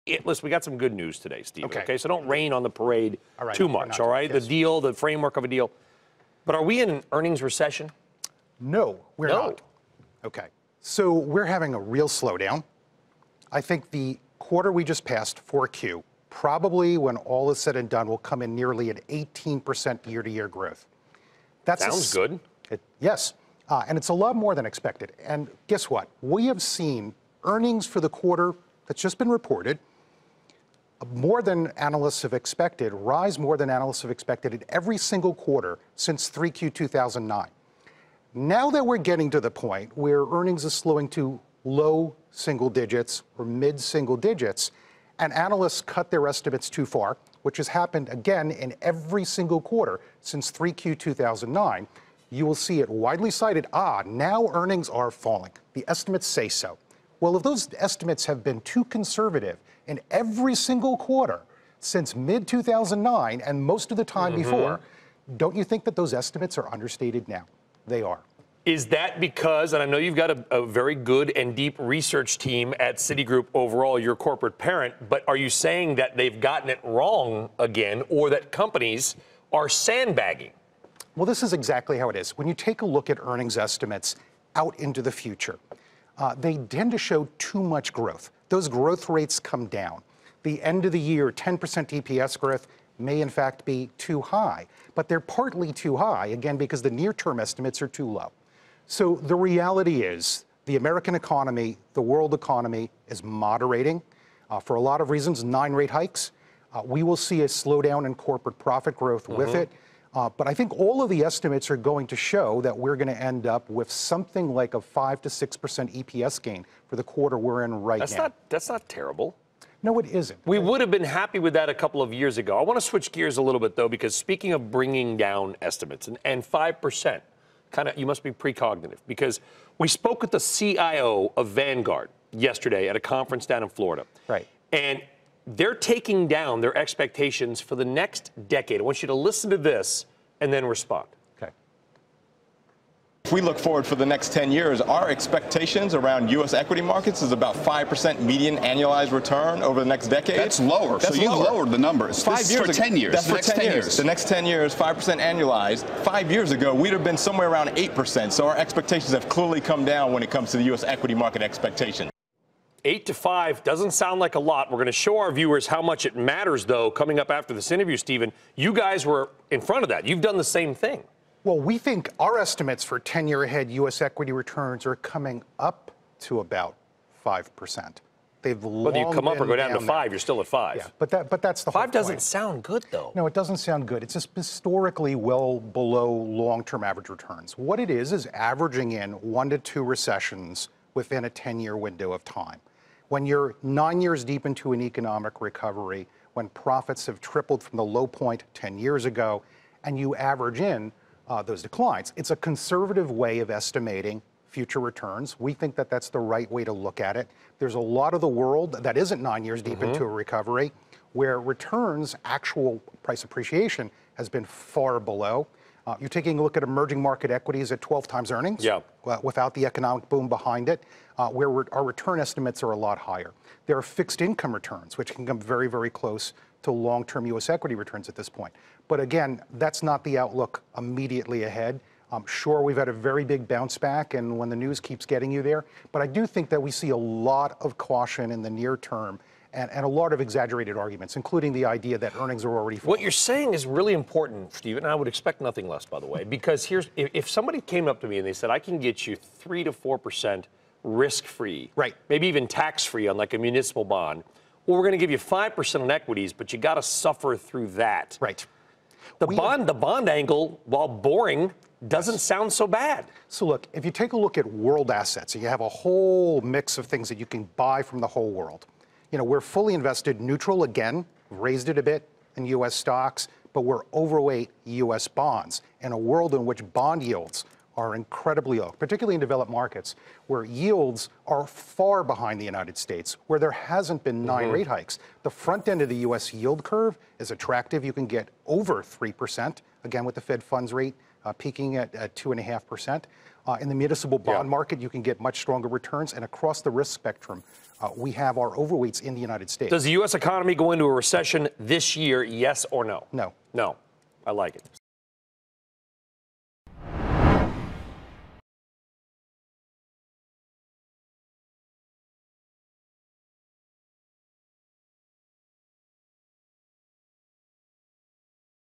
Markets cheer tentative shutdown deal
Steven Wieting of Citi Private Bank gives his take on markets and the DC drama.